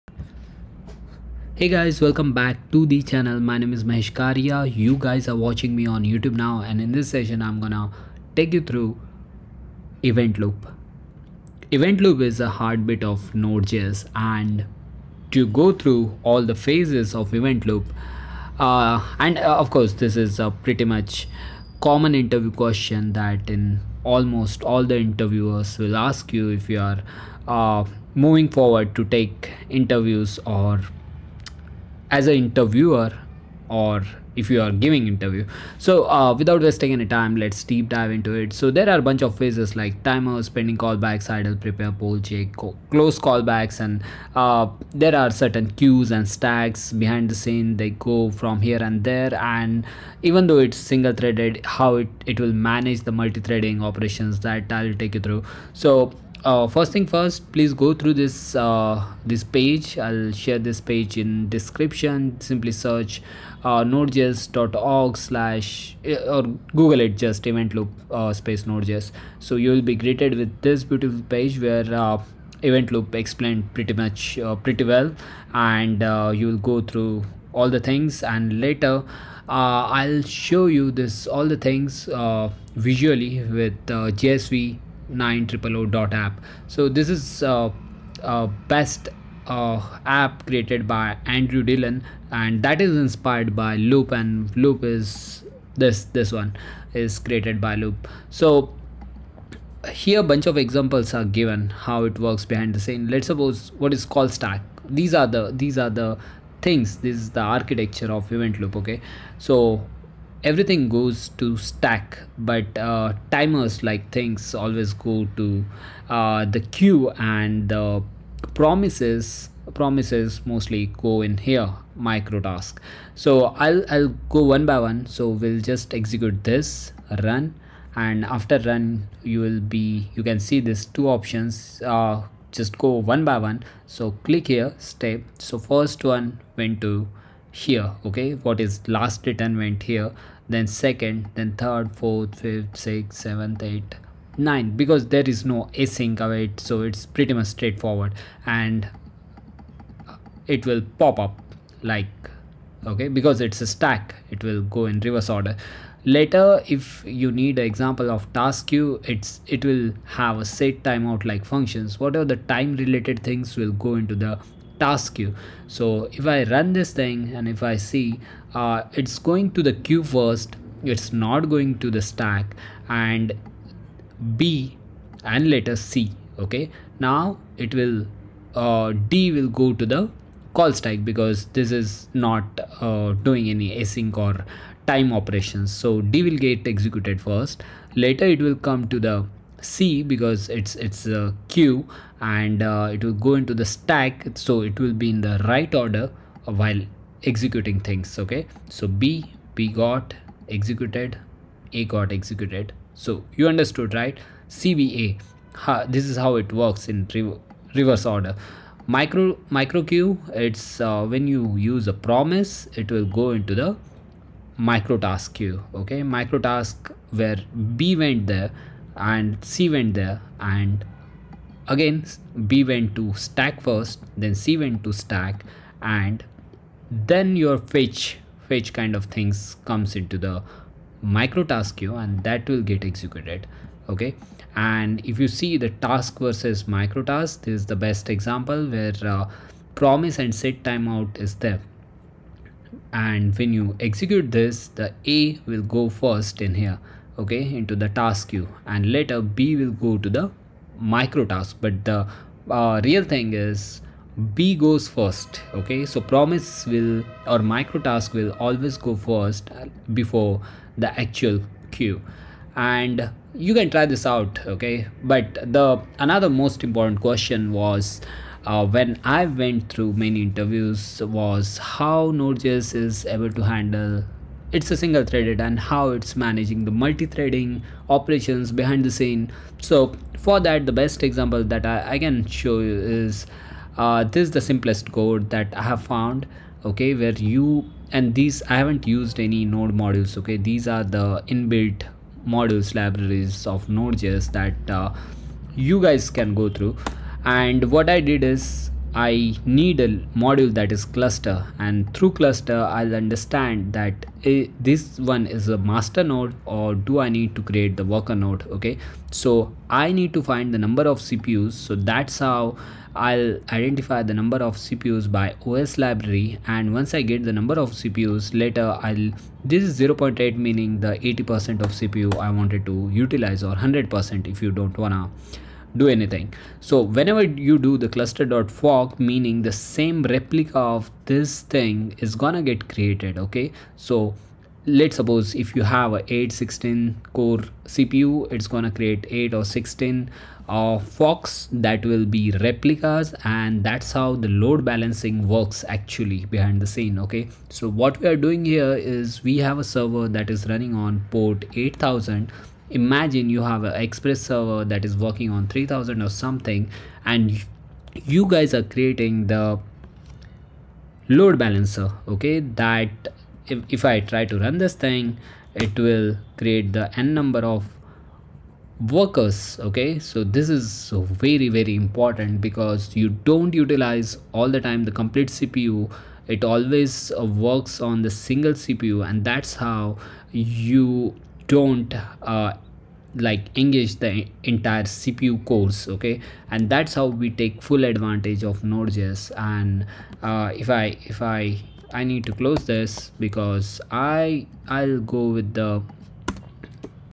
Understanding the Node.js Event Loop: Is Node.js Single or Multithreaded?
Welcome to our deep dive into the Node.js Event Loop! In this video, we uncover the mechanics of how Node.js handles asynchronous operations, despite its single-threaded nature. Learn how Node.js utilizes the Event Loop alongside the Libuv library to perform non-blocking I/O operations, which allows it to efficiently manage multiple tasks concurrently. We'll explore:

🌀 The architecture of the Node.js Event Loop
🧵 Single-threaded nature vs. Multi-threading capabilities with Worker Threads
🛠️ Practical examples to demonstrate asynchronous vs. synchronous execution
Whether you're a beginner looking to understand the fundamentals or an experienced developer aiming to refine your knowledge of Node.js internals, this video will provide valuable insights into how Node.js achieves high performance in real-world applications.

Find me anywhere by Username: @itsmaheshkariya